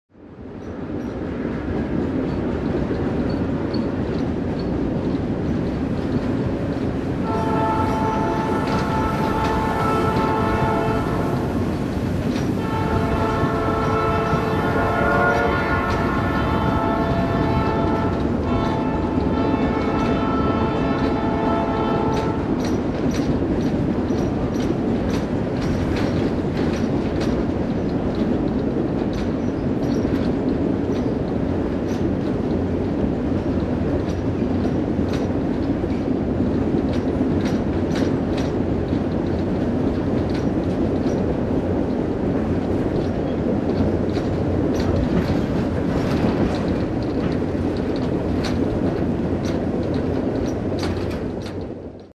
Reading & Northern Runby - Oneida Clipper At Miller's Crossing
The Reading & Northern's 'Oneida Clipper' excursion performs one more runby at Miller's Crossing Road near Port Clinton, PA on May 5, 2018. Rather than stand with the crowd on the ground to shoot the train, I stood on the train to shoot the crowd on the ground - a 'reverse runby', of sorts.